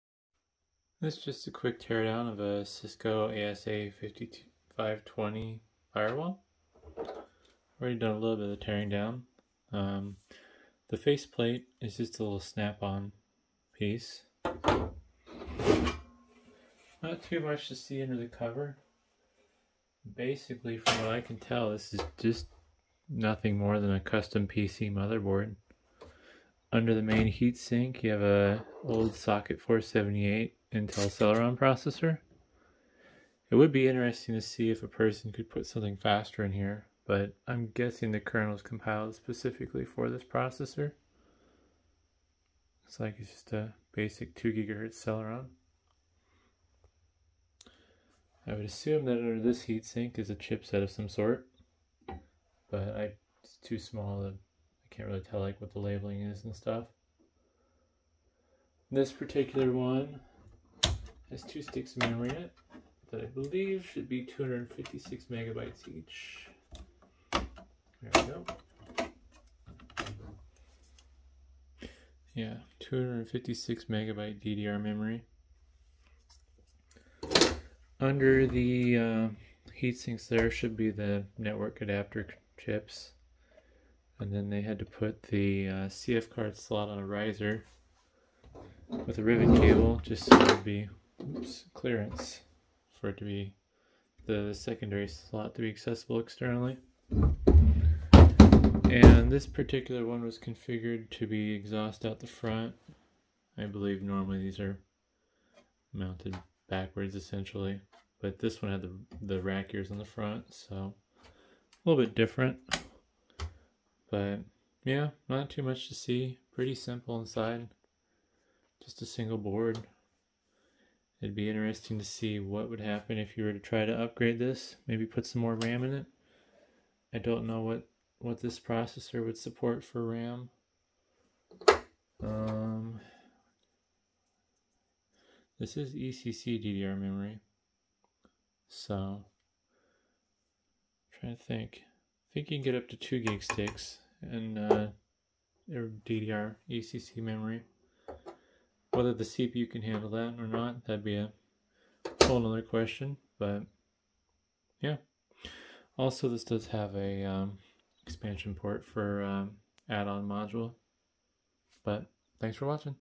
Cisco Adaptive Security Appliance ASA 5520 Firewall
It's basically a custom PC motherboard inside.  Wonder if a person could easily upgrade it or even play doom on it?

Parts for these firewalls if they are still in stock:

Want to support what I do?